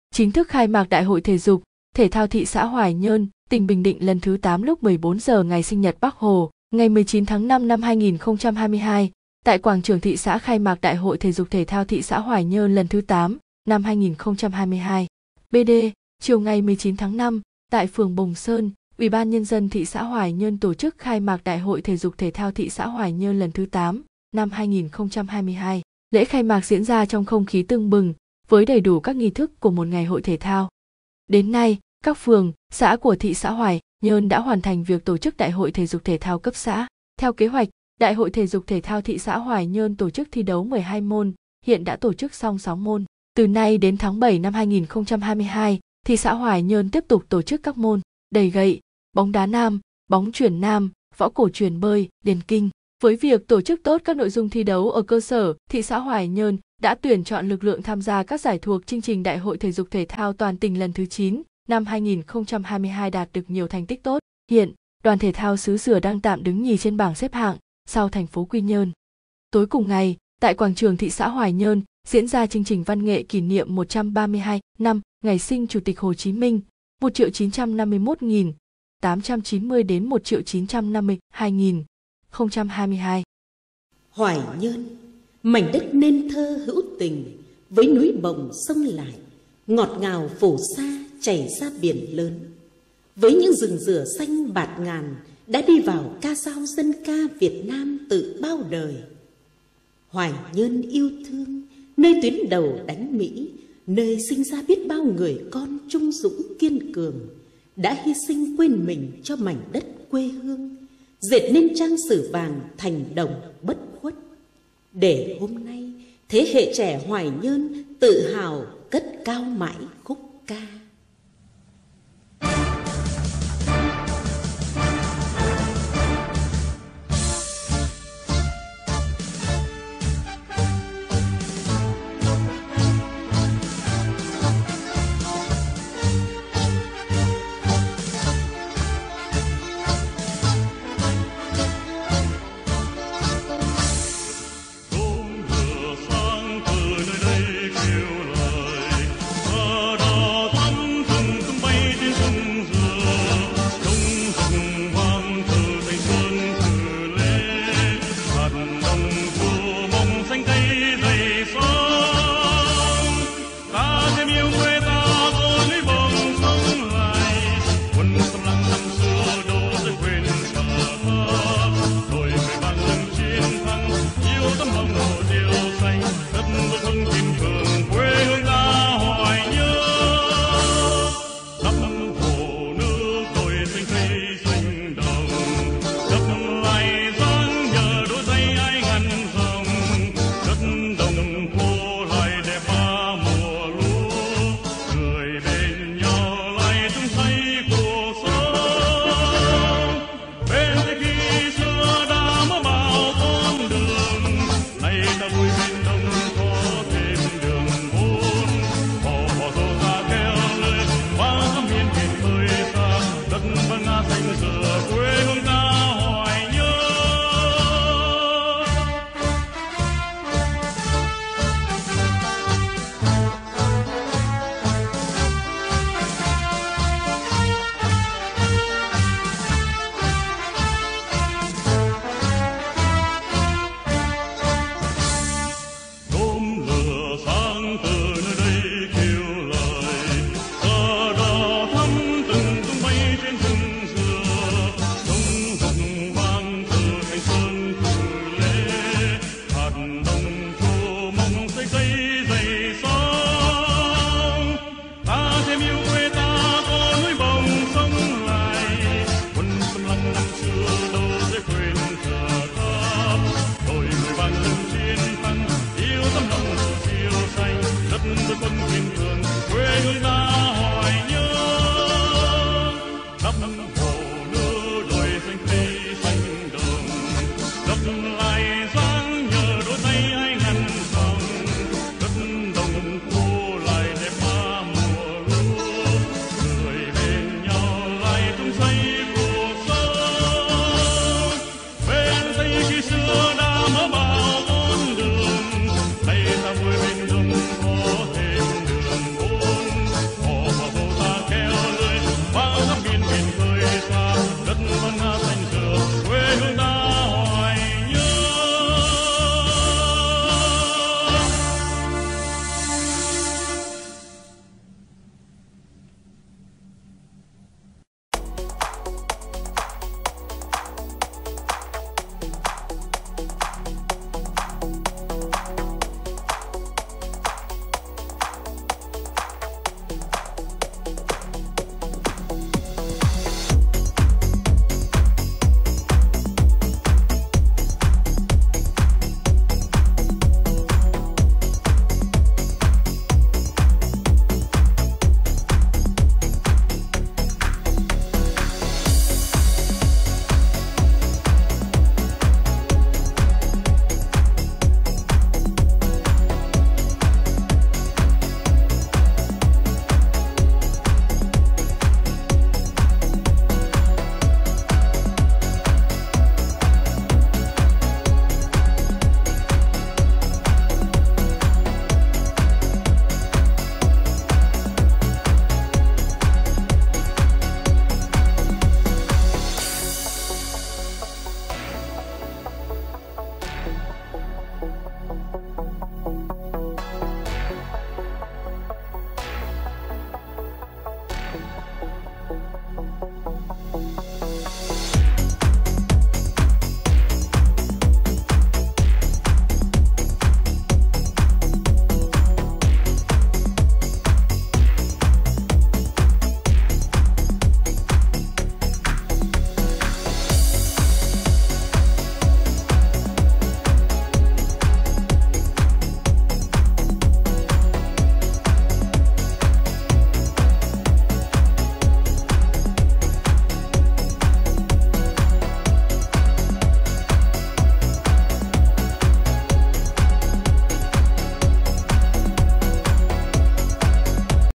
Khai mạc Đại hội Thể dục-Thể thao thị xã Hoài Nhơn, tỉnh Bình Định lần thứ VIII 19/5/2022
Khai mạc Đại hội TDTT TX Hoài Nhơn lần thứ VIII - năm 2022
(BĐ) - Chiều 19.5, tại phường Bồng Sơn, UBND TX Hoài Nhơn tổ chức khai mạc Đại hội TDTT TX Hoài Nhơn lần thứ VIII - năm 2022. Lễ khai mạc diễn ra trong không khí tưng bừng, với đầy đủ các nghi thức của một ngày hội thể thao.
Đến nay, các phường, xã của TX Hoài Nhơn đã hoàn thành việc tổ chức đại hội TDTT cấp xã. Theo kế hoạch, Đại hội TDTT TX Hoài Nhơn tổ chức thi đấu 12 môn; hiện đã tổ chức xong 6 môn. Từ nay đến tháng 7.2022, TX Hoài Nhơn tiếp tục tổ chức các môn: Đẩy gậy, bóng đá nam, bóng chuyền nam, võ cổ truyền bơi, điền kinh. Với việc tổ chức tốt các nội dung thi đấu ở cơ sở, TX Hoài Nhơn đã tuyển chọn lực lượng tham gia các giải thuộc chương trình Đại hội TDTT toàn tỉnh lần thứ IX - năm 2022 đạt được nhiều thành tích tốt. Hiện, đoàn thể thao xứ Dừa đang tạm đứng nhì trên bảng xếp hạng, sau TP Quy Nhơn.
• Tối cùng ngày, tại Quảng trường TX Hoài Nhơn diễn ra chương trình văn nghệ kỷ niệm 132 năm Ngày sinh Chủ tịch Hồ Chí Minh (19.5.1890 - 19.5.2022).